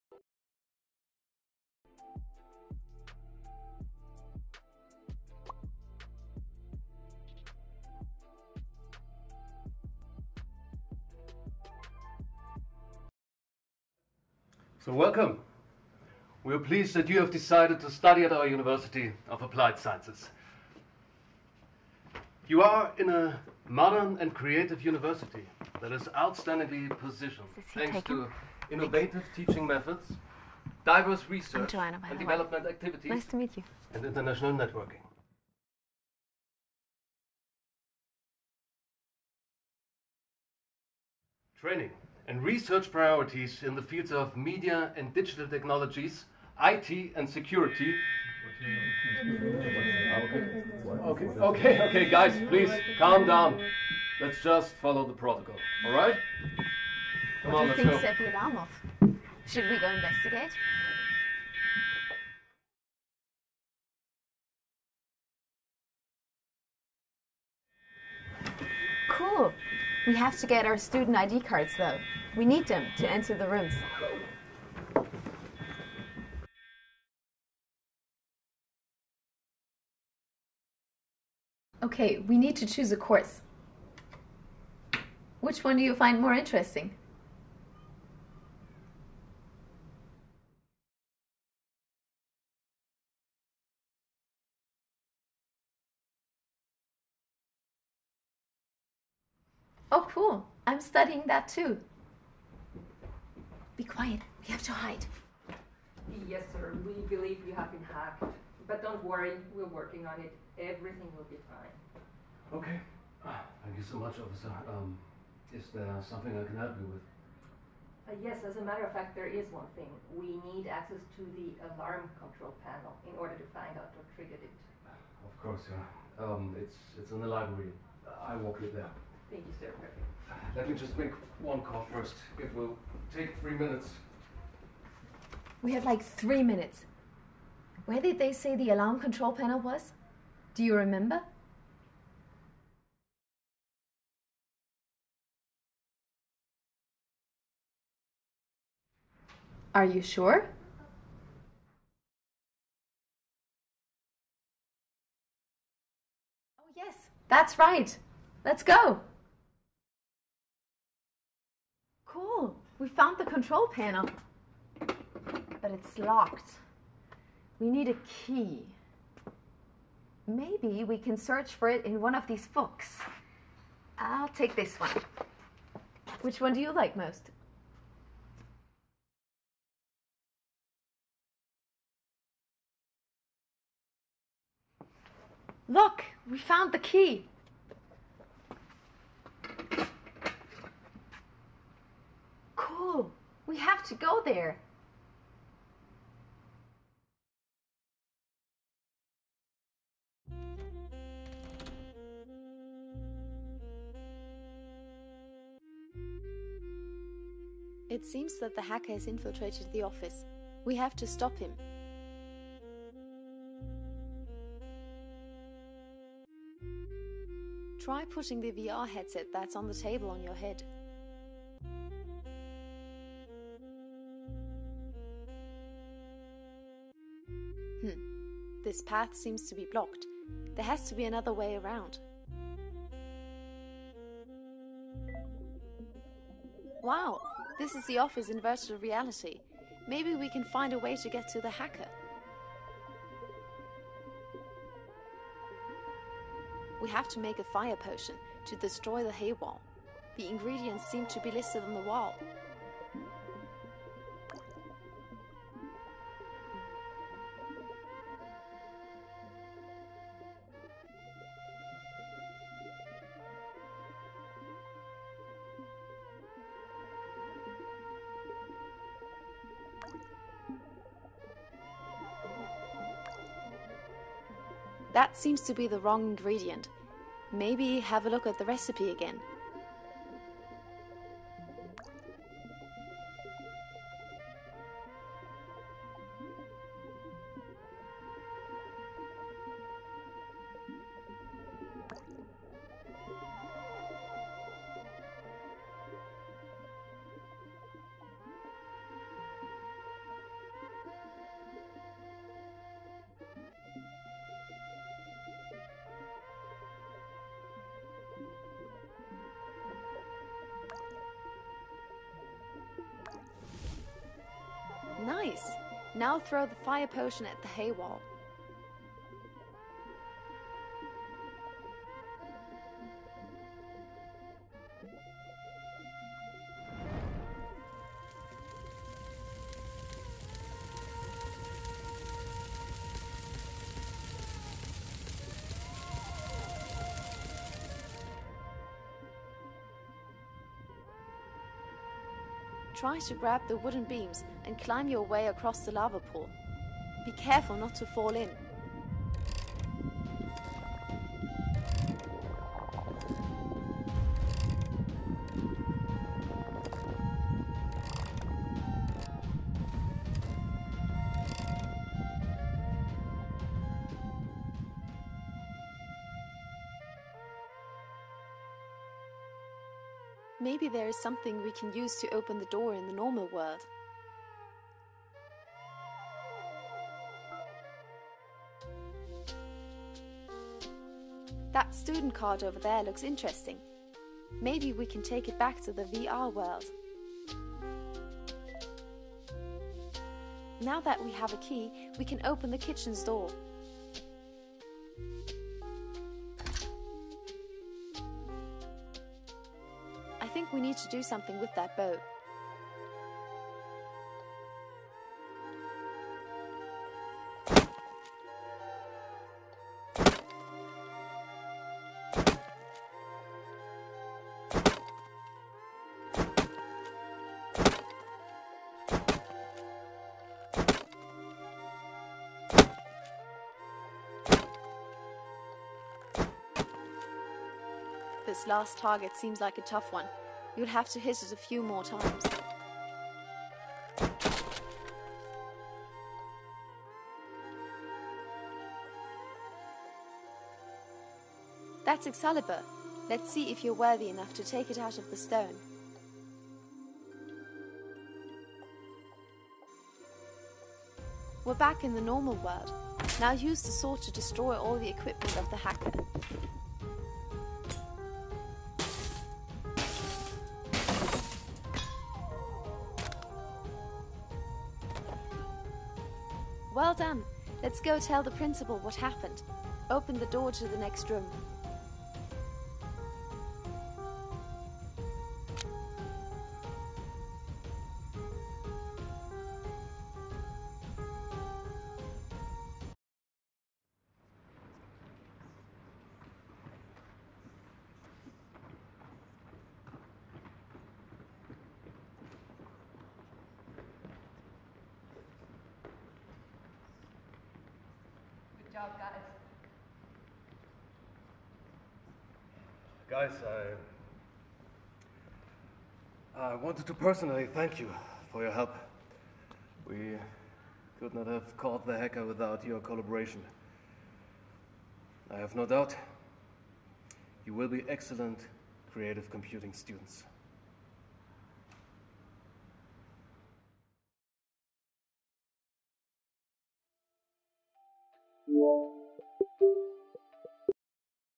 VREPS - The First Day - Game Path
The virtual experience "The First Day" was made by the VREPS team at the FHSTP during our European Project Semester in the summer semester of 2022. Our project combines a 360° interactive short film with virtual reality minigames that is meant to introduce new students to the environment and the studies that the Fachhochschule at St. Pölten has to offer.

In this particular path, the user has chosen for the course that focusses on gaming concepts and game design. The storyline and ending will be different depending on what the user chooses during the interactive short film. To view the other paths and endings that our possible in our experience, check out more videos on my channel.

Made by Ruth Fernandez, Marco Vander Gracht, Timo Baginski, Tibo Van Houtte, Ermando Dulluku, Daniel Dickinson and Casper De Keyser.